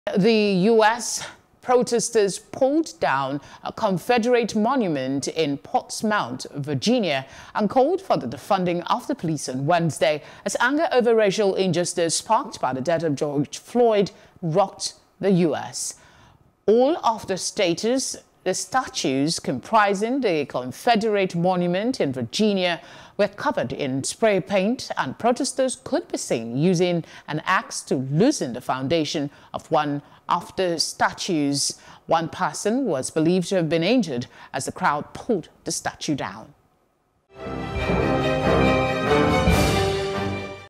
Protesters Topple Confederate Statue In The U.S.
And in the US...Protesters pulled down a Confederate monument in Portsmouth, Virginia and called for the defunding of the police on Wednesday as anger over racial injustice sparked by the death of George Floyd rocked the U.S.